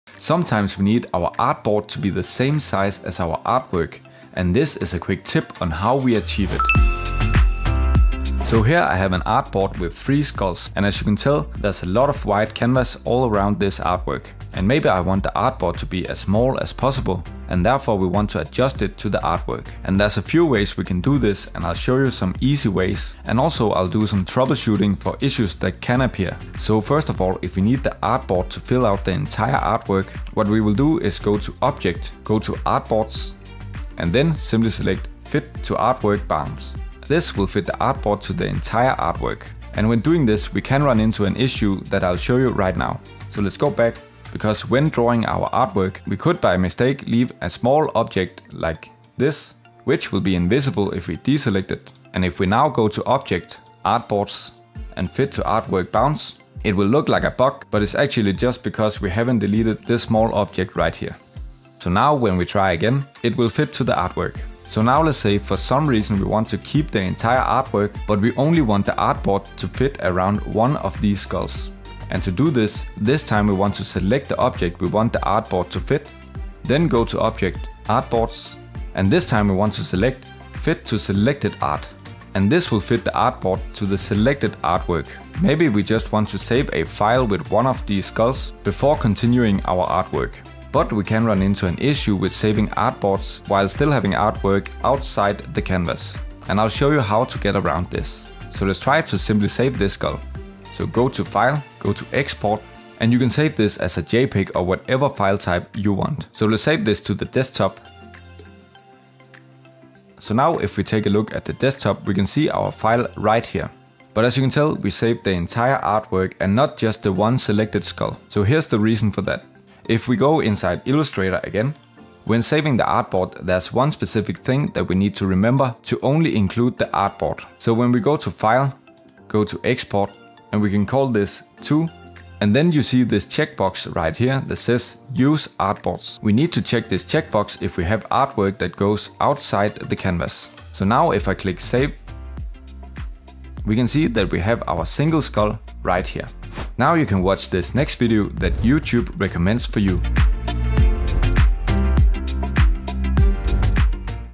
How To Fit Artboard To Artwork In Adobe Illustrator
A quick guide for how to fit artboard to artwork to avoid havnt too much empty space around your artwork. Also i will mention some issues that can appear and how to avoid these.

📒 Notes 📒

CTRL = Command
ALT = Option